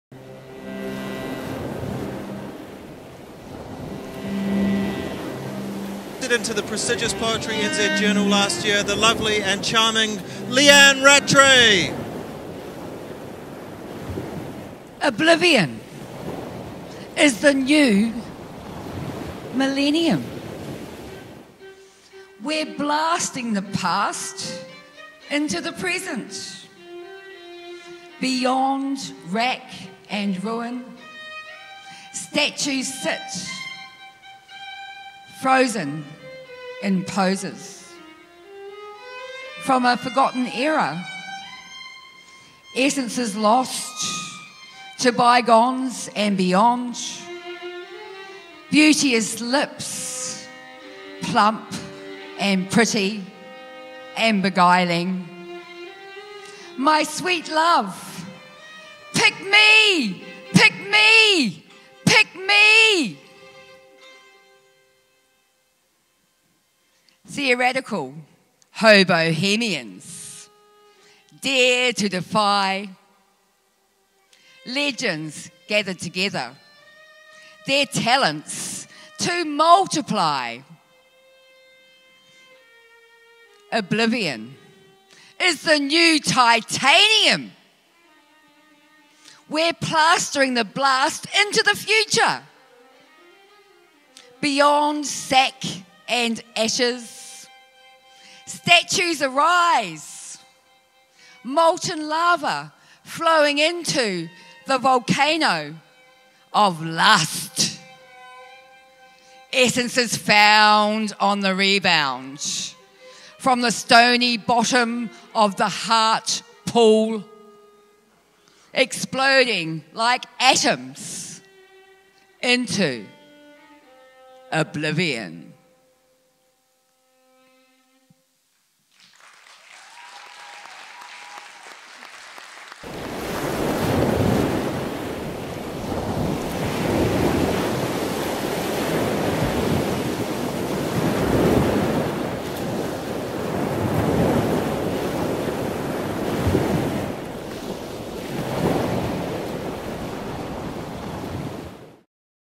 New Zealand Poet & Spoken Word Poet Leanne Rattray
Leanne Rattray Auckland, NZ Poet, performs her poem 'Oblivion'. She wrote this piece especially for the Theoradical Hobohemian's, a musical/poetry troupe. The show was performed at the Mercury Theatre, in the red light area of the city. This beautiful Edwardian theatre has had its ups and downs but it is still a stunning theatre . Auckland Graffiti Artist Christoph Gould painted a canvas to create the 'Love & Oblivion' visual to compliment her poem.